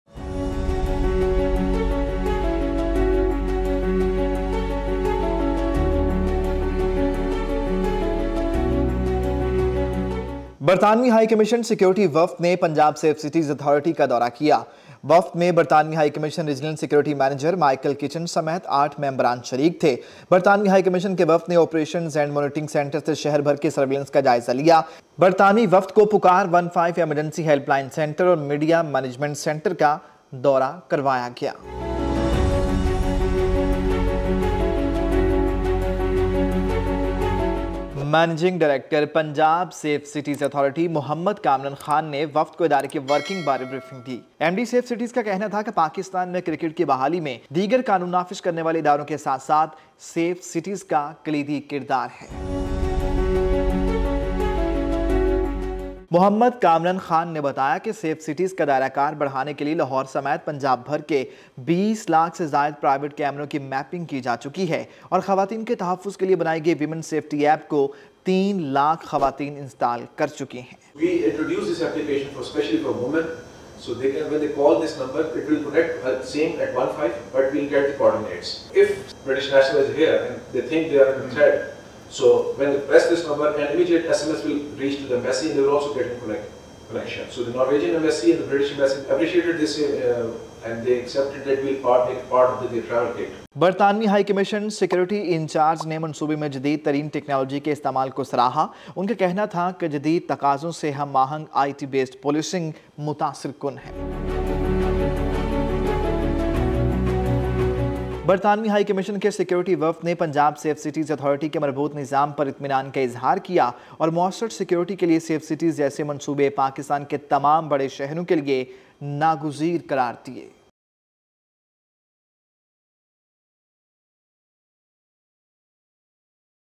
British High Commission Delegation Visits Punjab Safe Cities Authority
Discover the significant security collaboration as the British High Commission security delegation explores Punjab Safe Cities Authority. In this video, Managing Director Muhammad Kamran Khan provides a comprehensive briefing on the institution's operational framework. Gain insights into the delegation's perspective on the indispensable role of the Safe Cities project for enhancing security effectiveness in Pakistan. Watch now for exclusive details and valuable insights